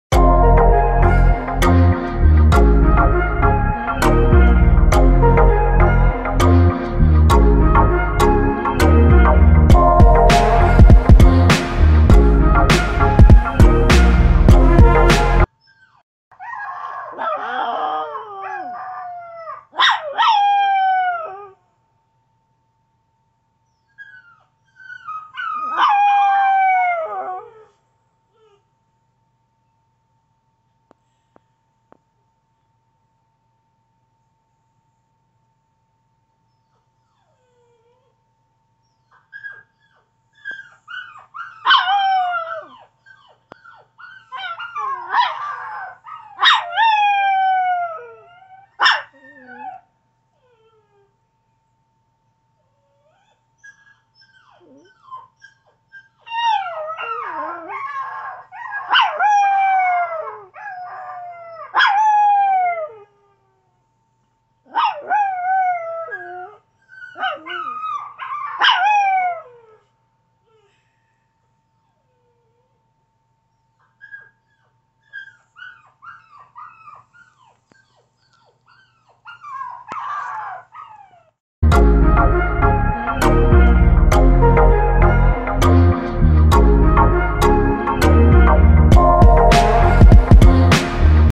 Chihuahua howling
This is a short video of our Chihuahua Chevy howling. Enjoy 😊